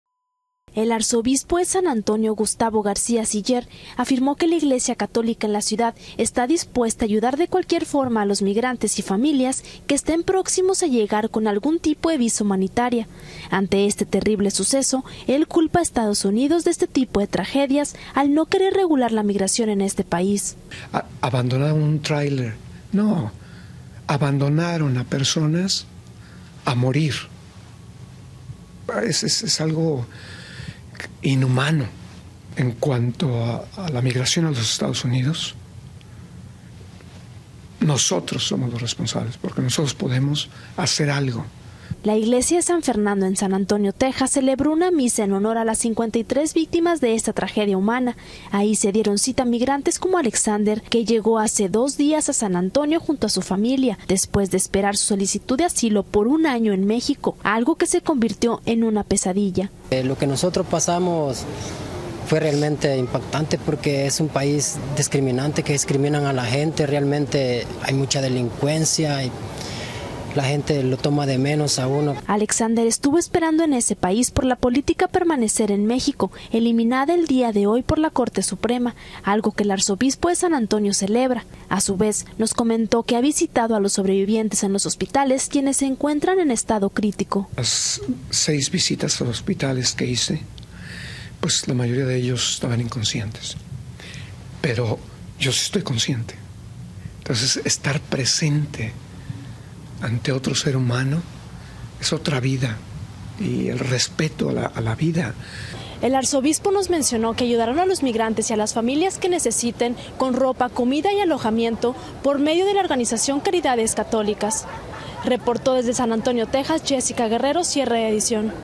Celebran misa en honor a migrantes fallecidos en San Antonio
Celebran una misa en honor a las víctimas de la tragedia migrante en San Antonio, Texas. La iglesia católica promete apoyar a los extranjeros recién llegados a Estados Unidos.